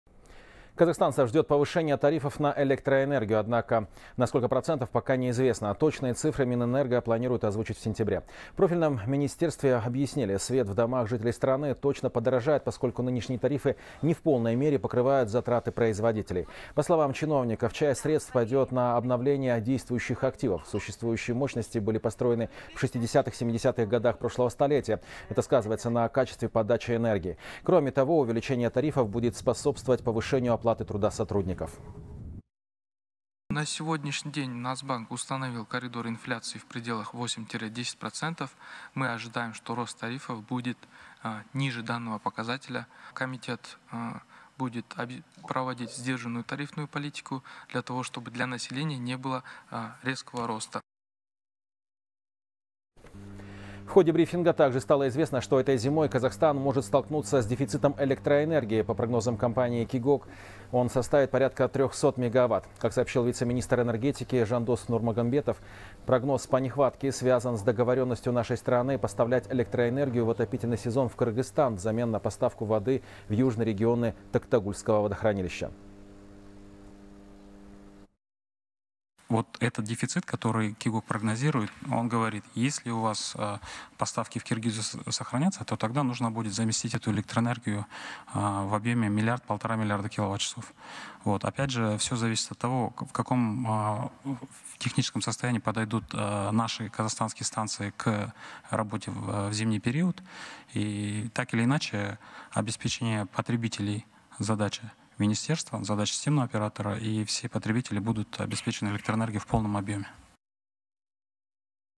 Повышение тарифов на электроэнергию ожидает казахстанцев
Казахстанцев ждёт повышение тарифов на электроэнергию. Однако на сколько процентов, пока неизвестно. Точные цифры Минэнерго планирует озвучить в сентябре, передает «Хабар 24».